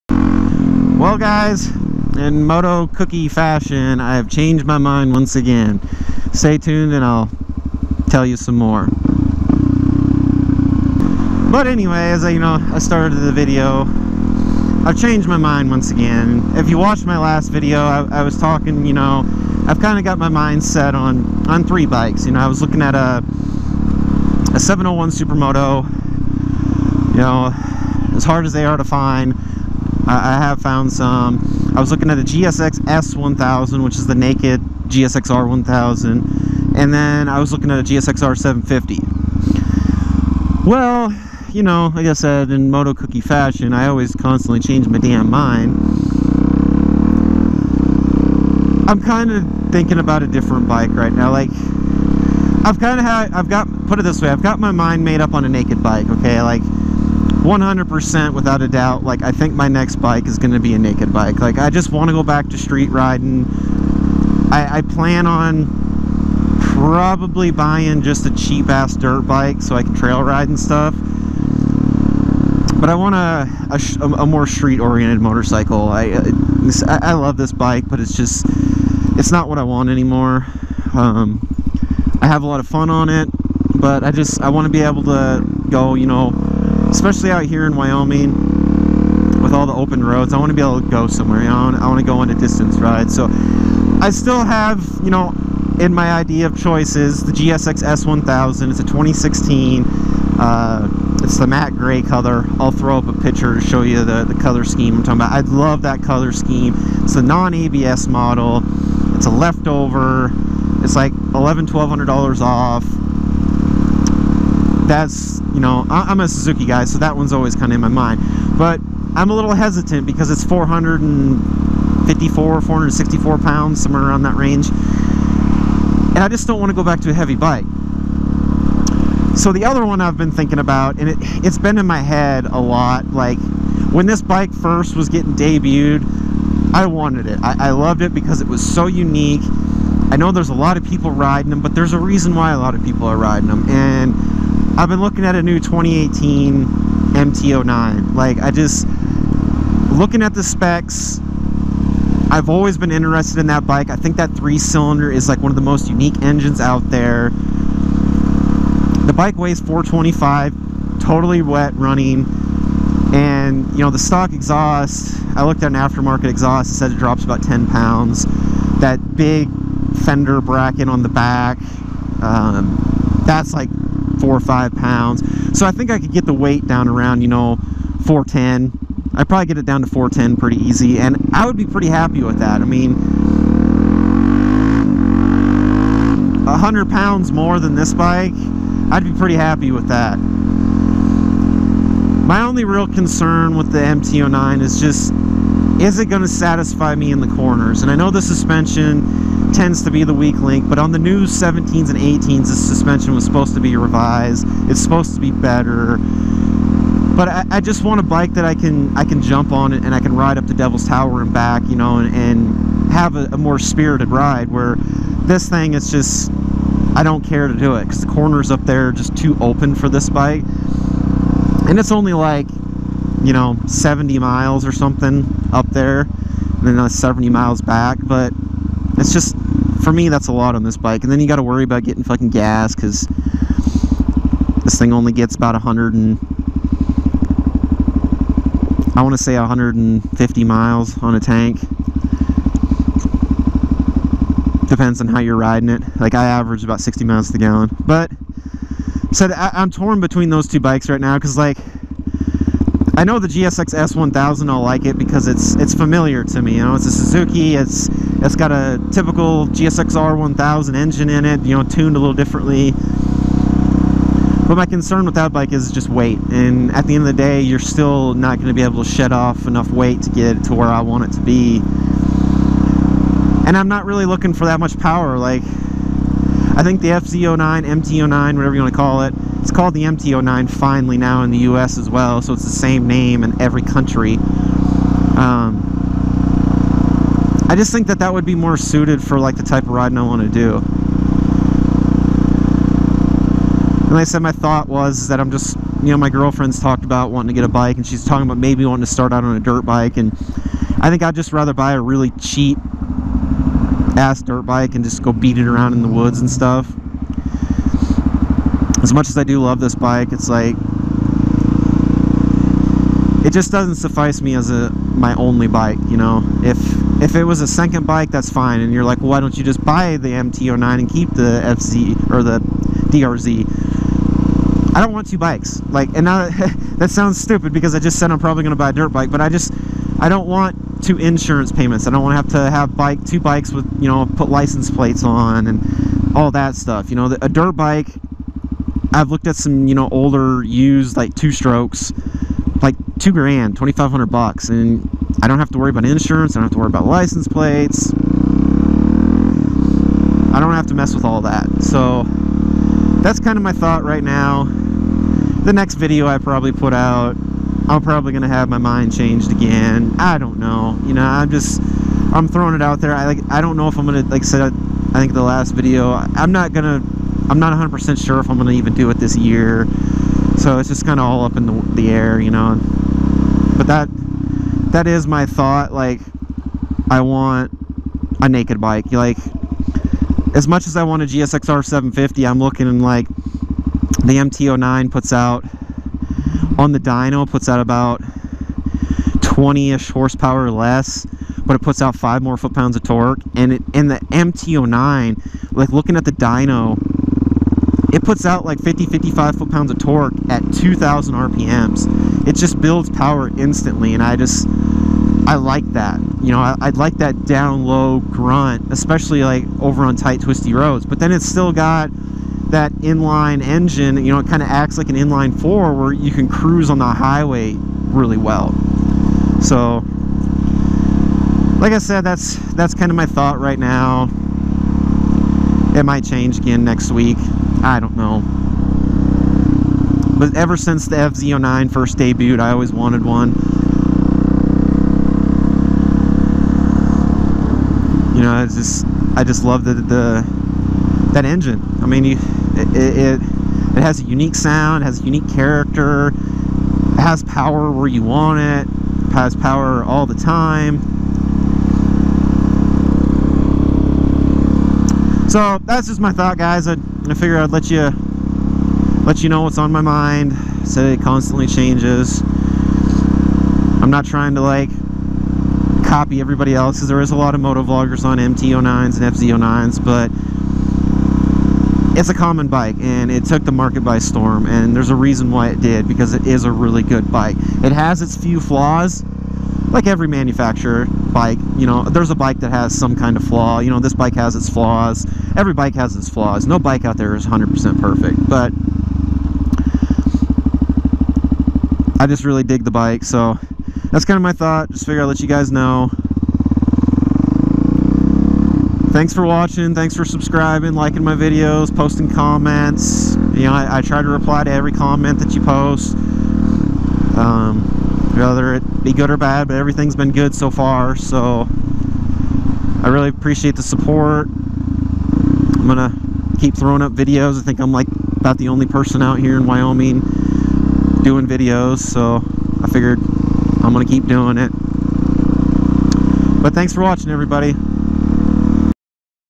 Changing My Mind..Again!
...Talking About Another Bike That Interests Me...

2016 Suzuki DRZ400SM "Leela"

Stickers Wish List

Gear:
GoPro Hero 5 Session